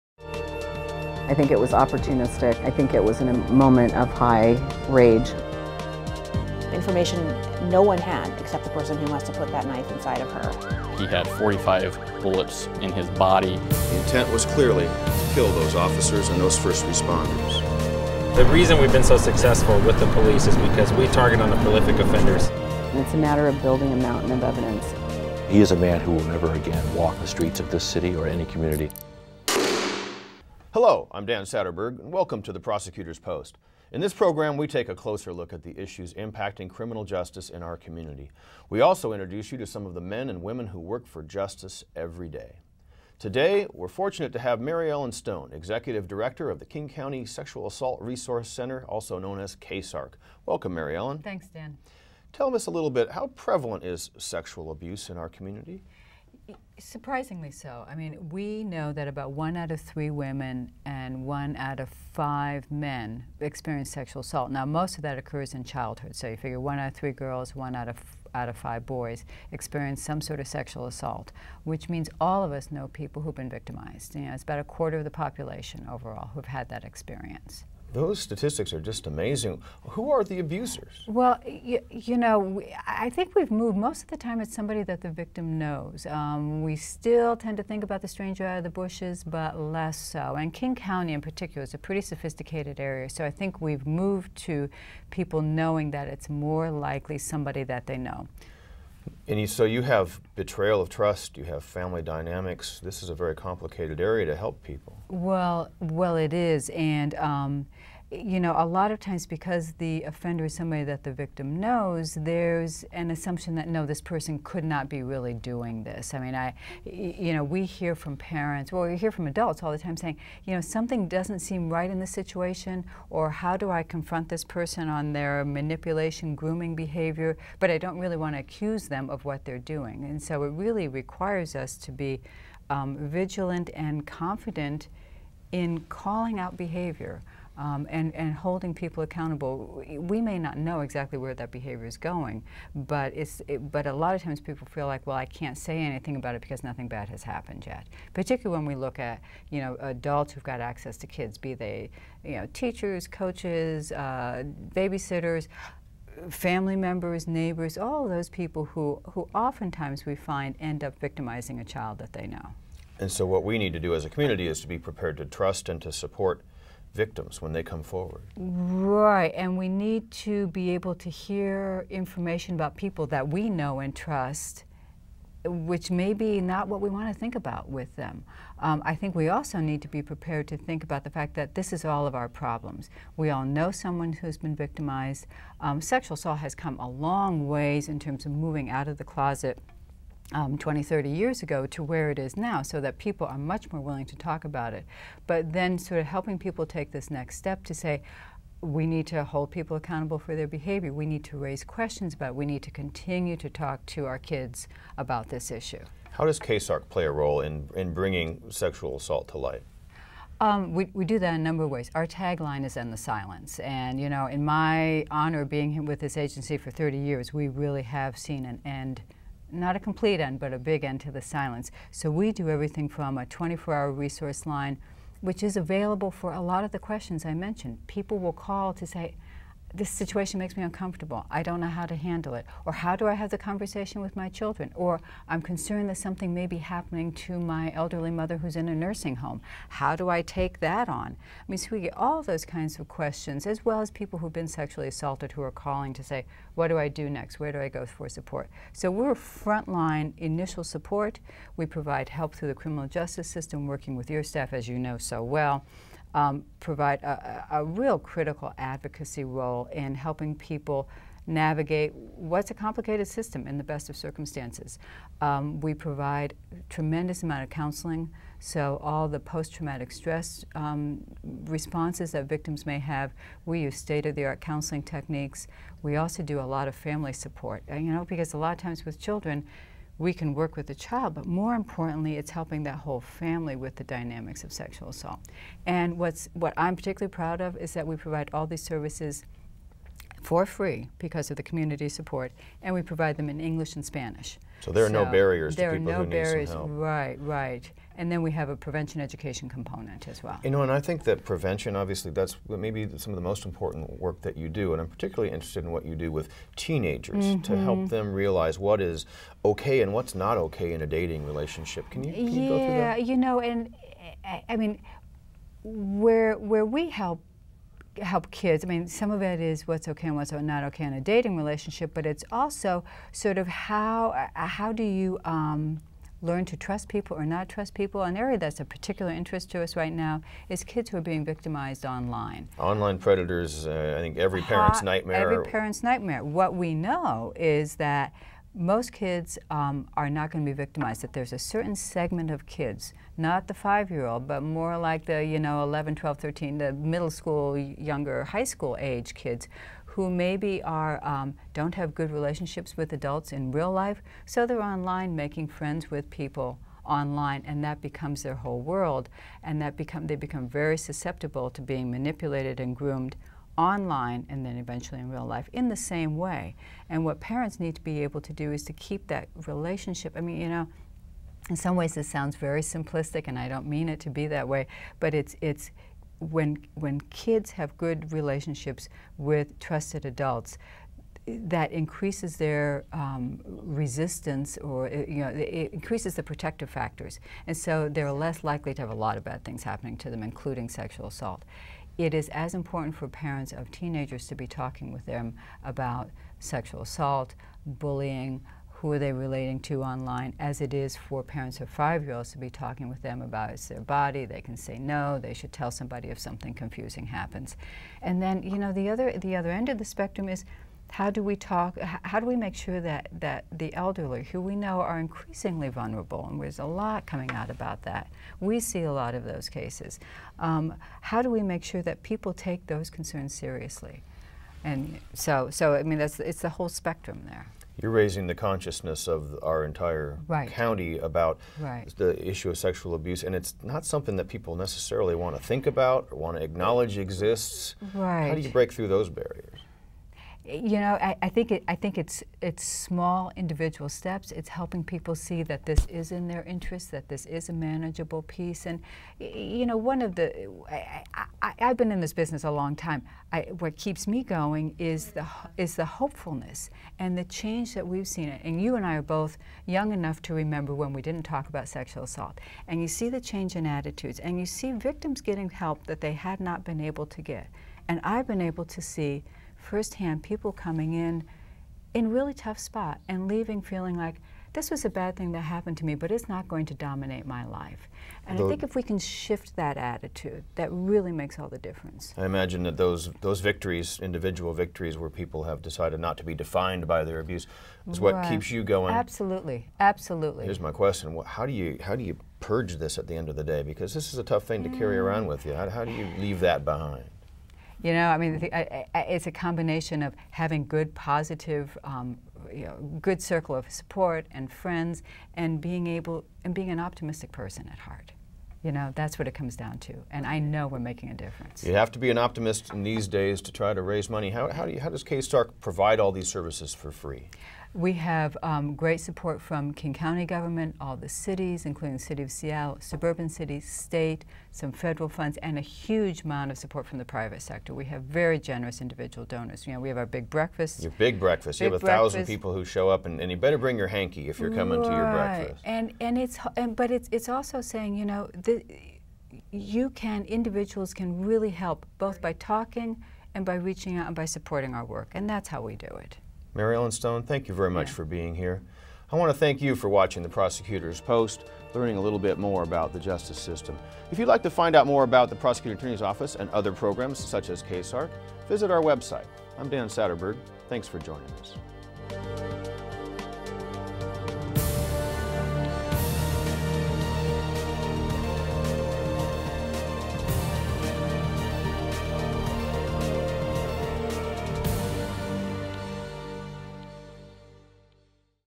Prosecutor's Post - Mary Ellen Stone
King County Prosecuting Attorney Dan Satterberg sits down with Mary Ellen Stone, Director of the King County Sexual Assault Resouce Center (KSARC).